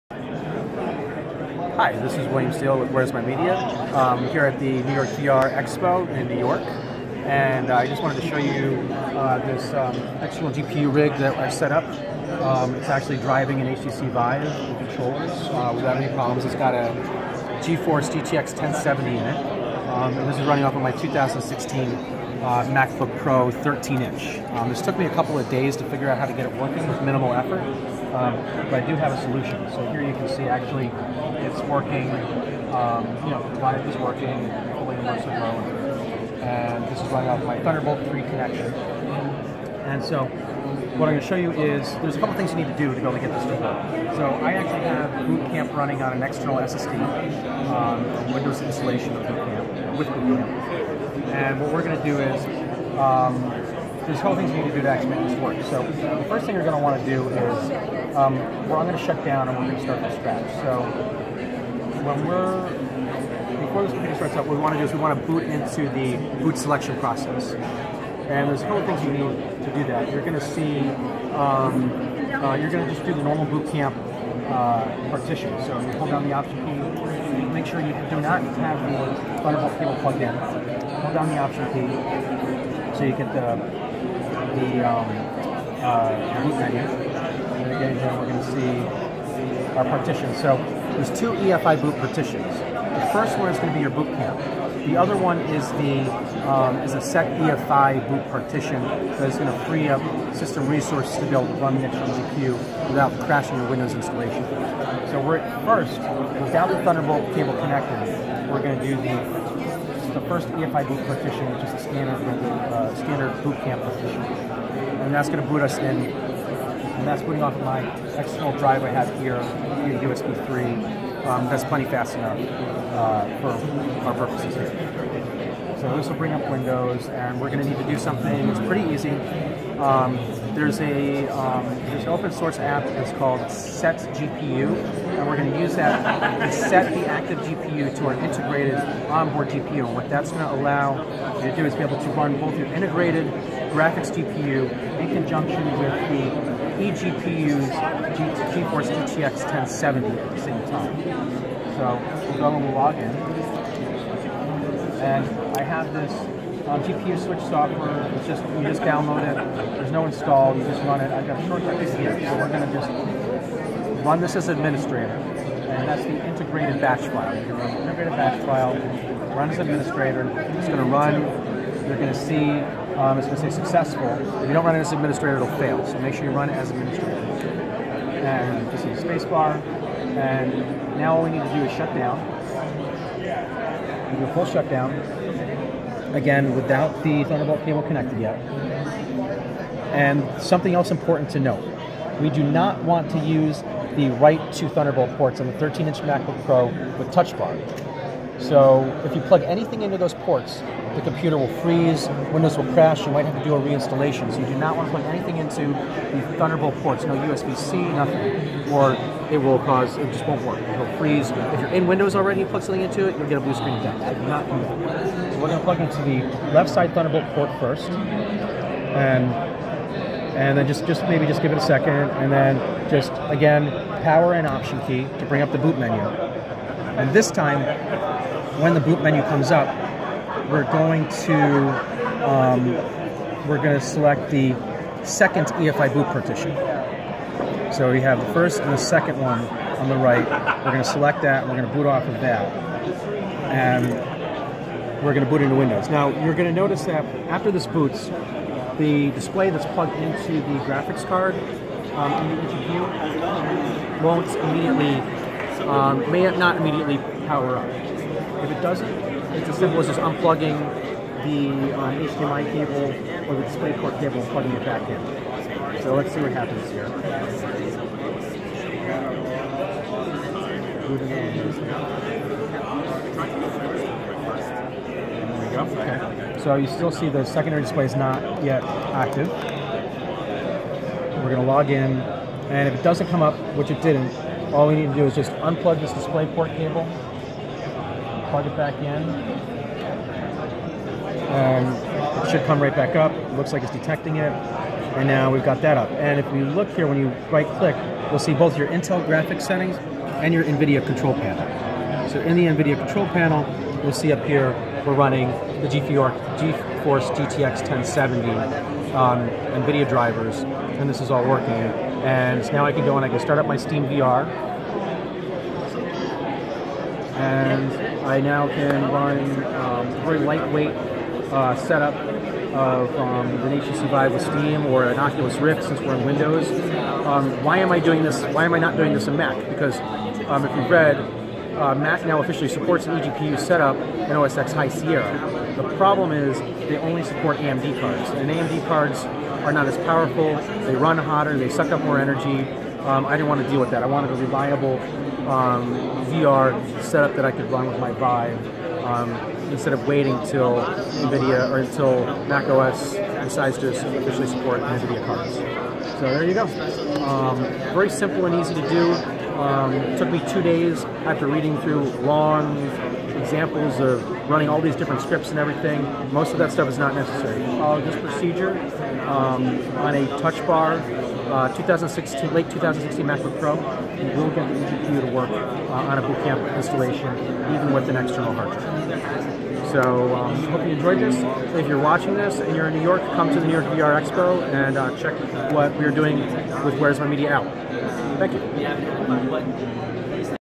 EGPU with Nvidia GTX 1070 on 13 inch Late 2016 MacBook Pro with Touchbar.
At the NYVR expo on Oct. 28th talking about my EGPU setup on my MacBook Pro.